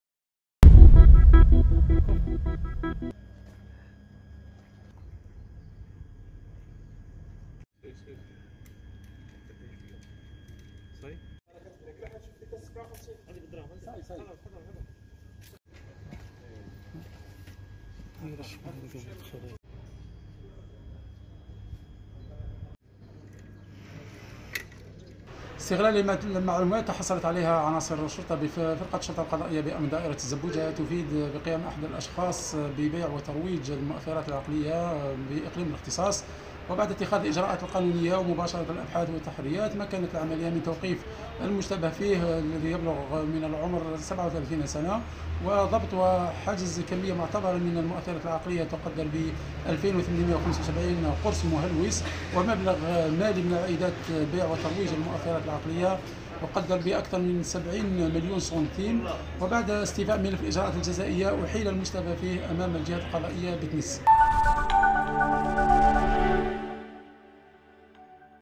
الشلف: حجز أزيد من 2800 قرص مهلوس
الشلف (وأج) - حجزت فرقة الشرطة القضائية بأمن دائرة الزبوجة (الشلف) أزيد من 2800 قرص مهلوس وأوقفت شخصا حسبما علم لدى ذات الهيئة الأمنية.